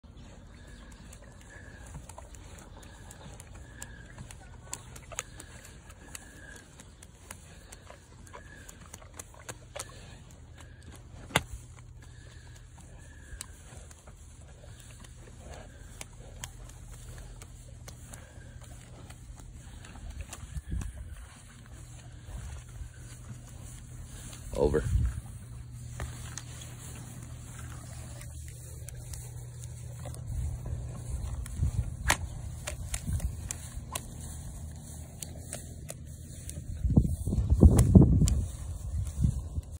DILIGENCE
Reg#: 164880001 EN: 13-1
Sire: Hawkeye
Dam: Coattails

Bred by Brice Conover
DILIGENCE was purchased at Belton 2021. I found DILIGENCE as a unique class winner there in being the extreme heavy structured boar that has a plenty of look. The part that set him ahead for me is when we make hogs as stout as DILIGENCE they normally don't have his athleticism and length of stride. His bones and feet are big. His body cavity looks like a 55 gallon drum. He walks square and has a herd boar skull. We may not be known as th